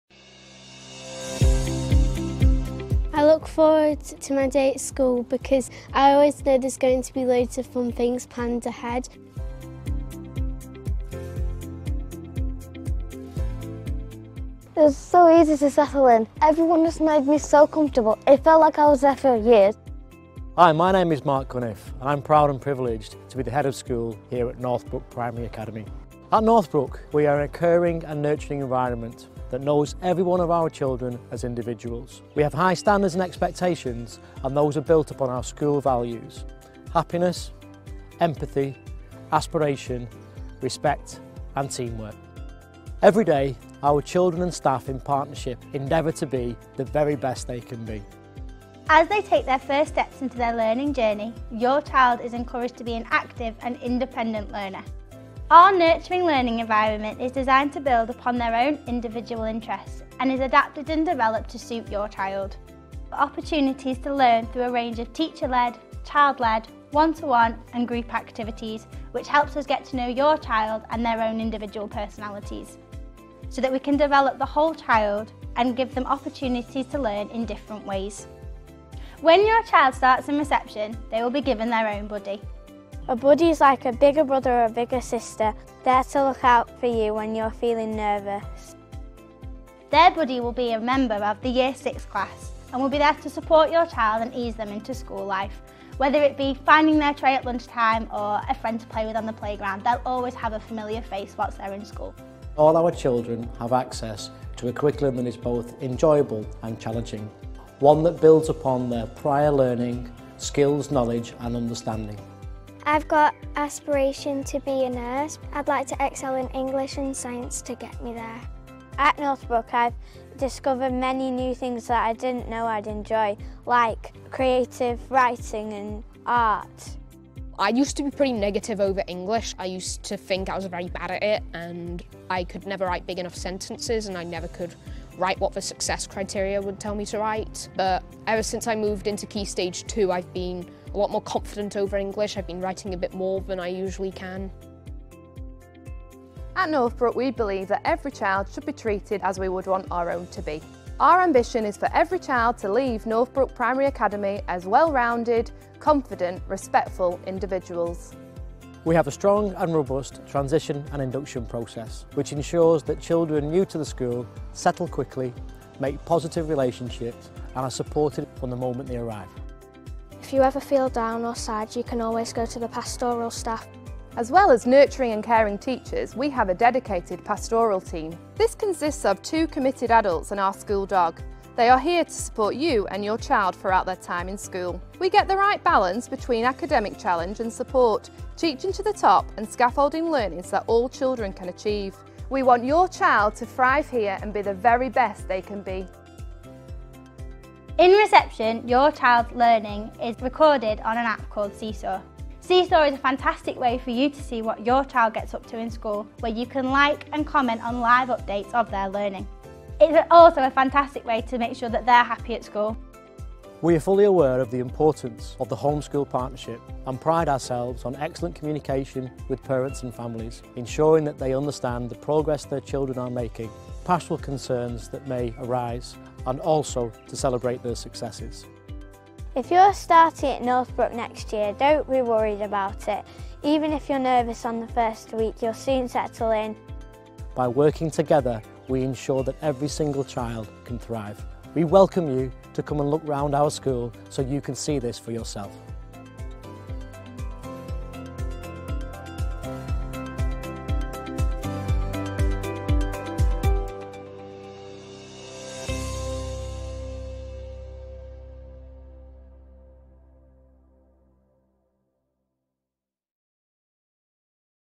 Northbrook Primary Academy - Leyland
So wish we had a school dog at Primary! The results of six hours filming in Leyland with the freshly rebranded 'Northbrook Primary Academy'.